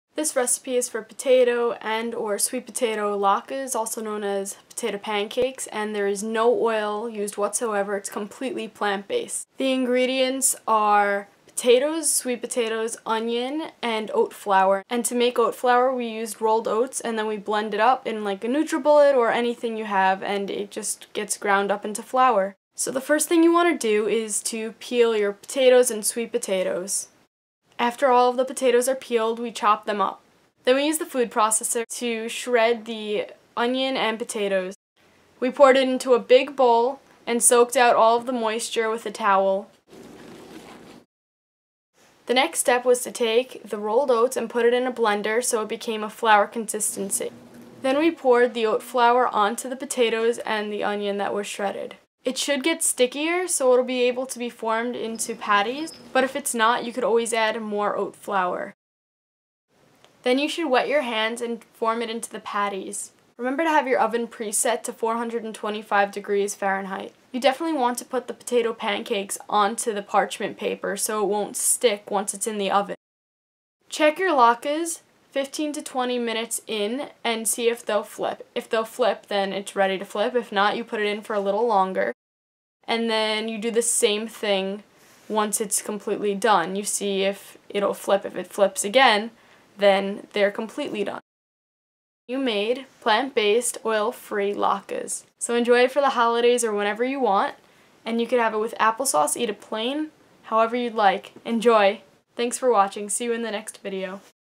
Plant Based Latkas Recipe - Potato Pancakes - No Oil
This is how you make Vegan Plant Based Latkas.  Happy Holidays!